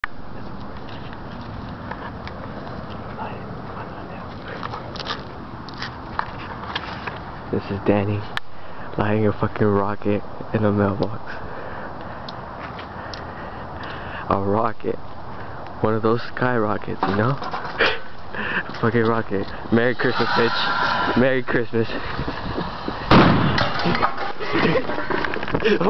Mailbox Boom
danny baby blowing up a mailbox haha